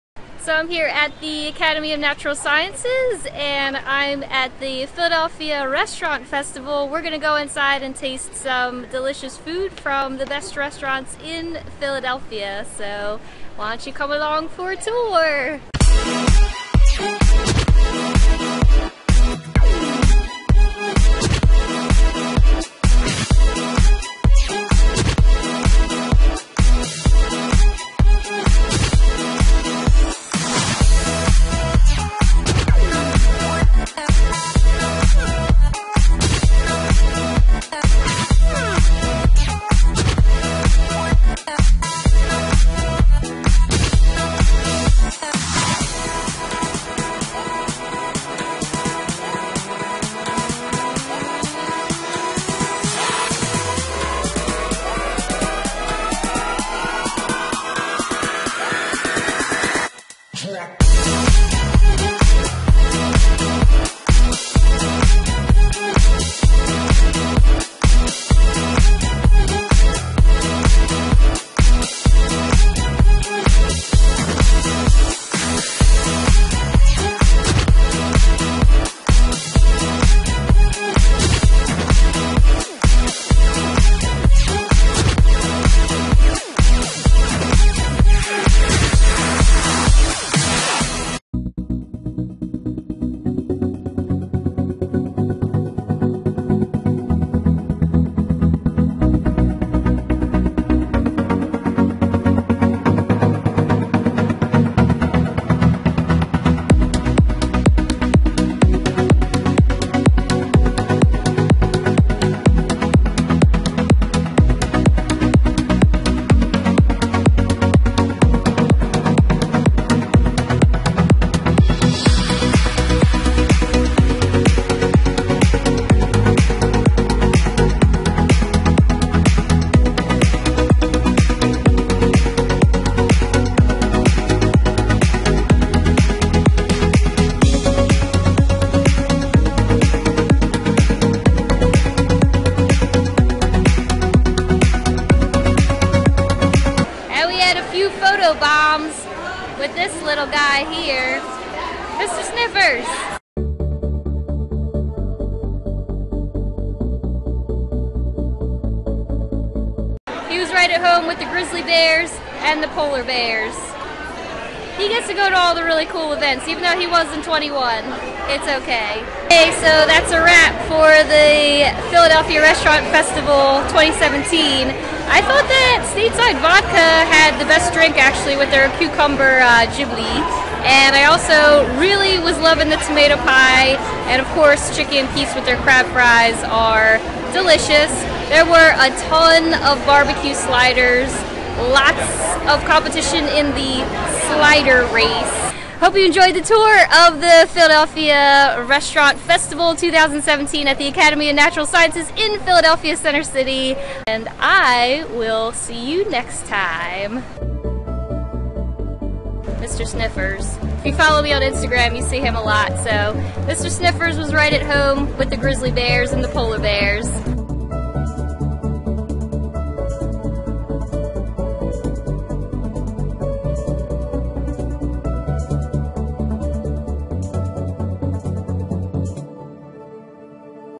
SKIP IT or GET TICKETS? Philadelphia Restaurant Festival 2017
The 2nd Annual Philadelphia Restaurant Festival held at the Academy of Natural Sciences in Philadelphia on Thursday June15th 2017 was a bit… underwhelming. Unless you are REALLY into drinking, the $50 price tag for the cost of admission, was not worth it Even though the venue was fun (I’m a huge fan of bears so I loved being at the museum) it was jam packed, hot and sweaty and the food was just okay. I give it a THREE out of FIVE stars (and that’s being generous!)

Although I do think there were a few things that stood out from the crowd, like Stateside Vodka, who I think had the best drink of the night (cucumber / vodka / club soda) and the tomato pie from Santucci’s was delicious, I was still disappointed with the rest of the food and the best I can say is that it was just mediocre. I really don’t think it showcased Philadelphia’s vibrant local restaurant scene and that’s a shame because there are so many awesome food destinations in Philly. Don’t get me wrong I LOVE supporting new up and coming food entrepreneurs in the Philly area, I just don’t think they brought their “A” game that night. The food was just an afterthought to the endless kiosks of booze. Side note: I do enjoy a well crafted cocktail, I’m just not a fan of an event that advertises to be a RESTAURANT festival when in fact, it should have been advertised as a BEVERAGE festival. Misnomer I suppose?

My recommendation is that you SKIP IT in 2018. That is unless, you JUST want to get your drink on and really don’t care that much about the quality of food – then by all means go for it! The $50 price tag will be a steal for you. But if you do want to experience an endless buffet of the finest Philly chefs in a fun environment, my two tops picks so far this year would be the Food for Thought Event (held at URBN sponsored by AchieveAbility) or the Philly Burger Brawl held at XFINITY LIVE. There’s more food festivals to be had this year so this list is not complete…

And just a reminder that this video is NOT AN AD. I was not paid or sponsored to do this vlog and all opinions are my own.

TakeItHome.com, ep. 39